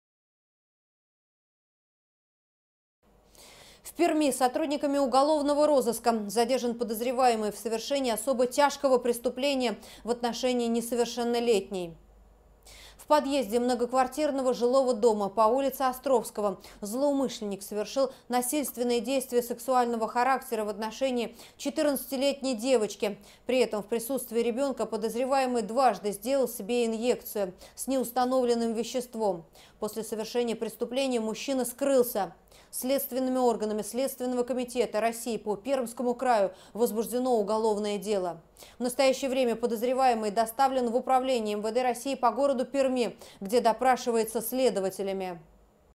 Задержан подозреваемый в совершении особо тяжкого преступления в отношении ребёнка
В Перми сотрудниками полиции задержан подозреваемый в совершении особо тяжкого преступления в отношении ребёнка.
11 ноября 2016 года в подъезде многоквартирного жилого дома по улице Островского злоумышленник совершил насильственные действия сексуального характера в отношении 14-летней девочки. При этом в присутствии ребенка подозреваемый дважды сделал себе инъекцию шприцем с неустановленным веществом. После совершения преступления мужчина скрылся.